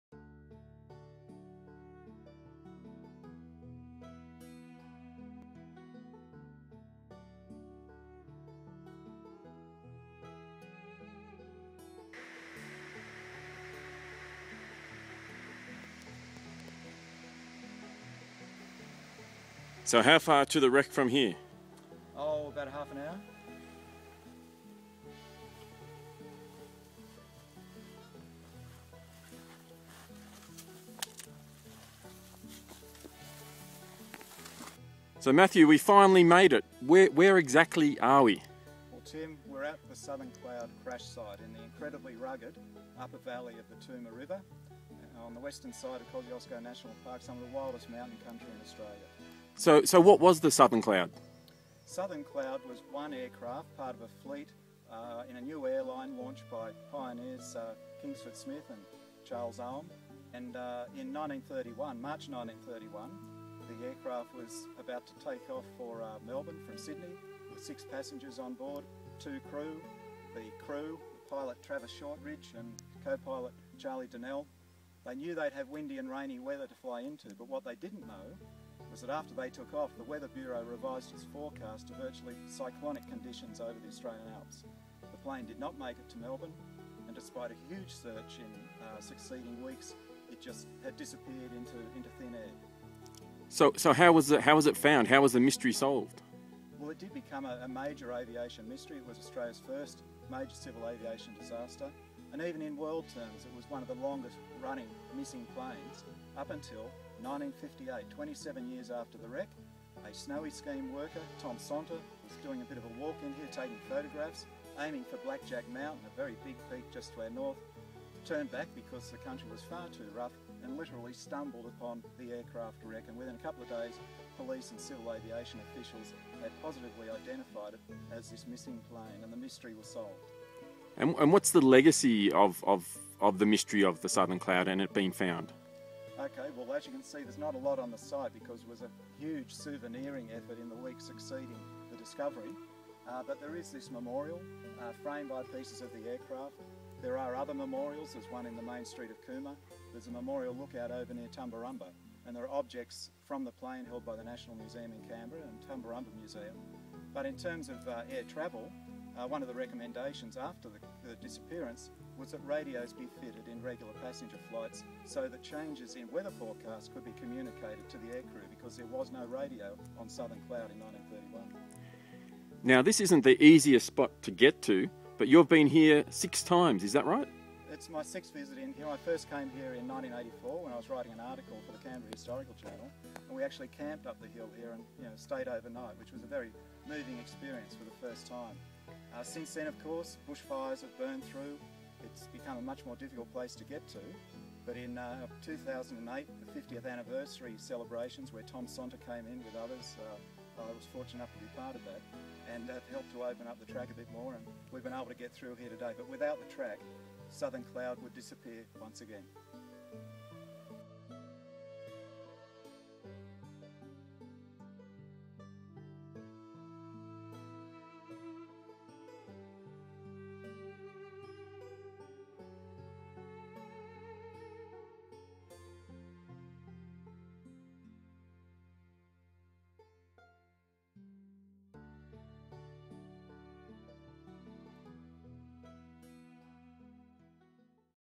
Tim the Yowie Man searches for the wreck of the Southern Cloud.
TYM heads out into the wilds of Southern New South Wales to discover the secret of the Southern Cloud.

Production: JGM64productions 2013.